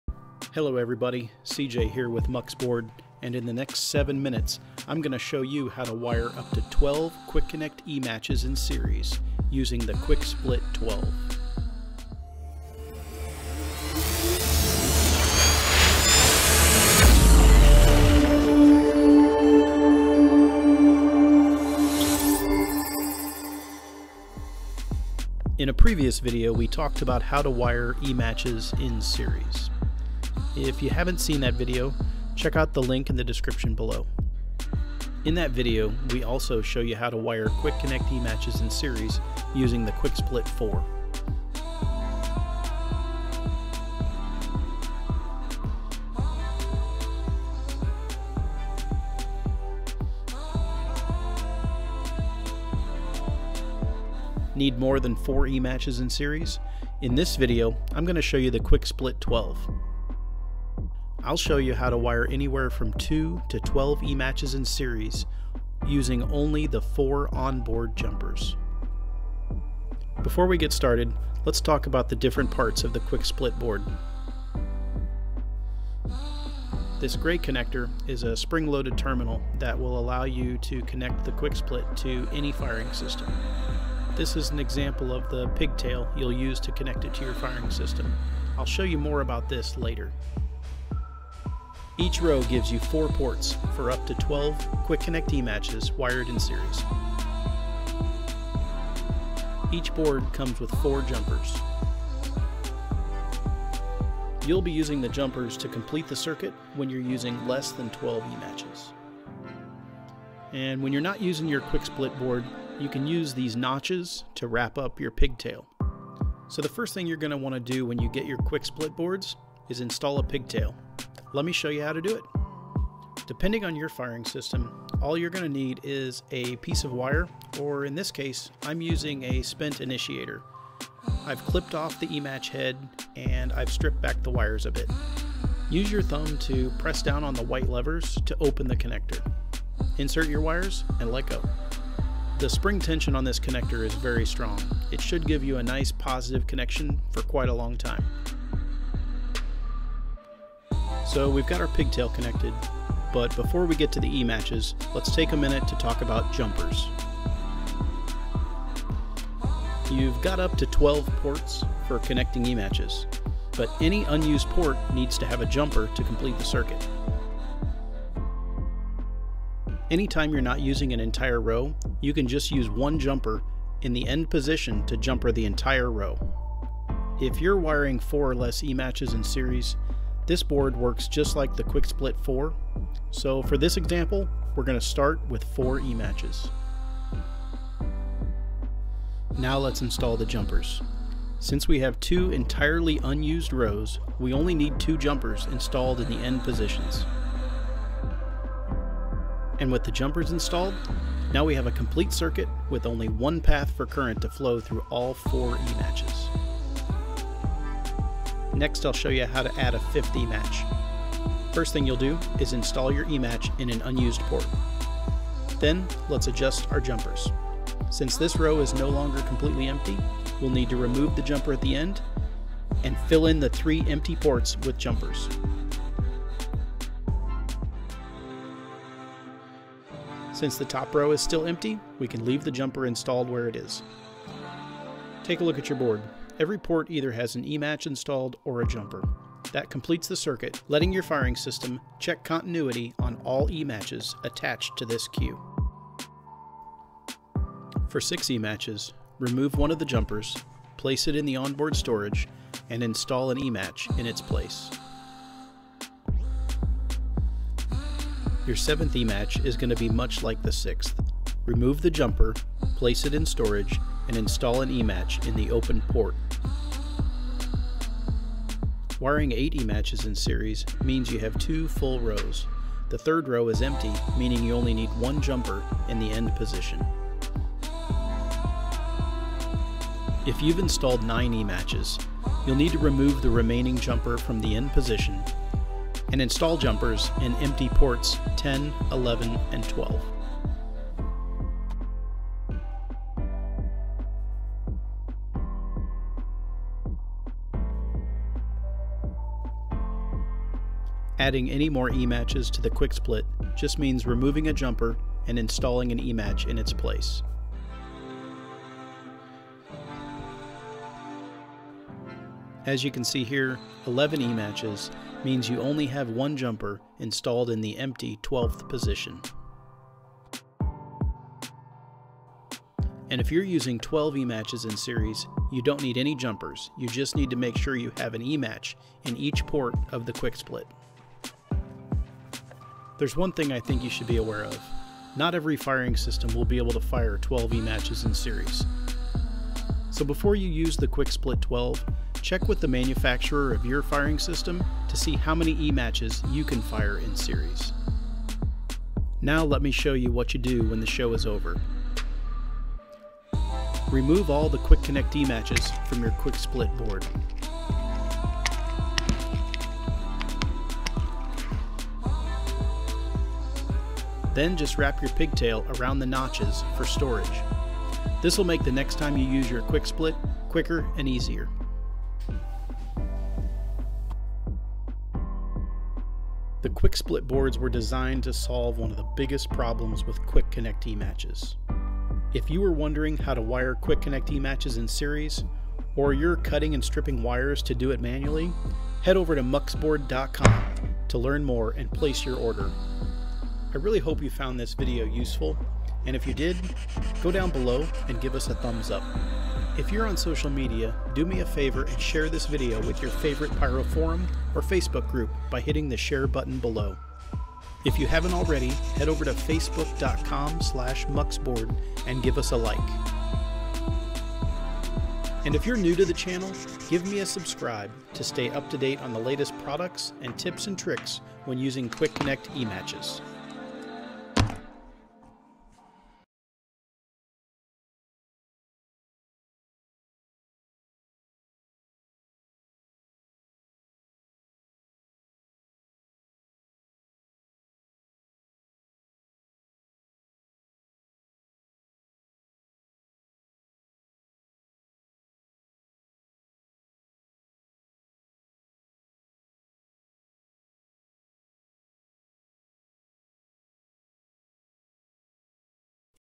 What is the QuickSplit-12?
My name is C. J. with muuxBRD.com, and today I'm going to show you the QuickSplit-12.  I'll show you how to wire up to 12 quick connect ematches in series in just seconds!  The QuickSplit will connect more ematches in series in less time than any other method.